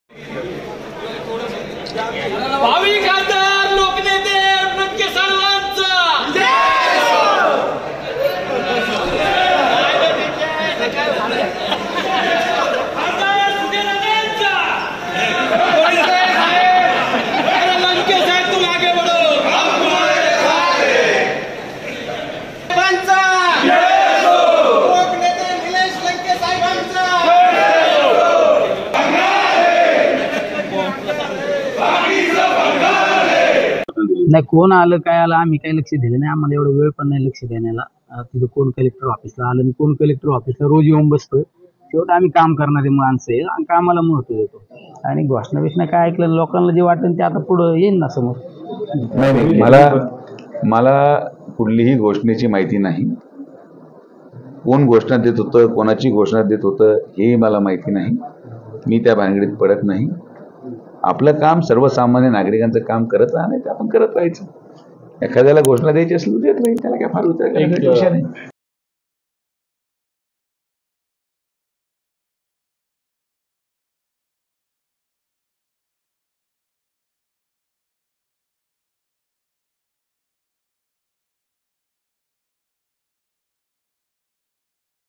Sujay Vikhe on Nilesh Lanke | लंकेंनंतर आता कार्यकर्त्यांनीही डिवचलं, विखे म्हणतात | LetsUpp Marathi
अहमदनगरच्या जिल्हाधिकारी कार्यालयात खासदार सुजय विखे आणि राष्ट्रवादीचे आमदार निलेश लंके आमने-सामने आल्याचं पाहायला मिळालंय.